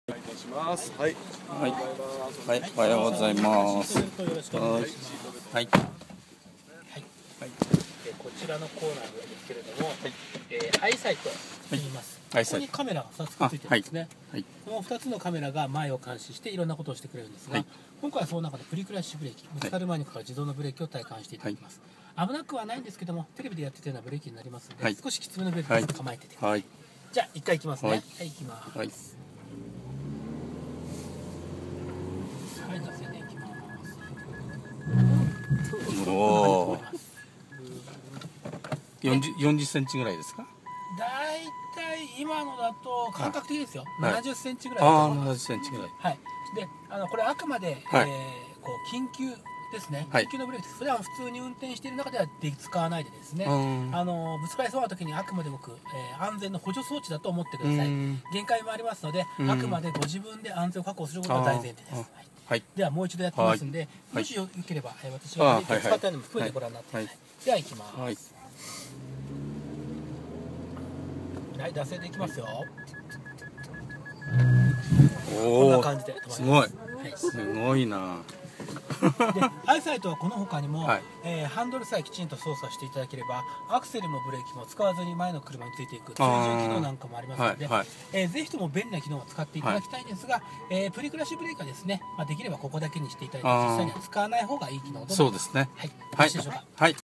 SUBARU XV 試乗の様子 1
SUBARU  XV Eyesight試乗の様子です‼‼  お見苦しいところがありますご勘弁下さい‼‼
    by  motoprin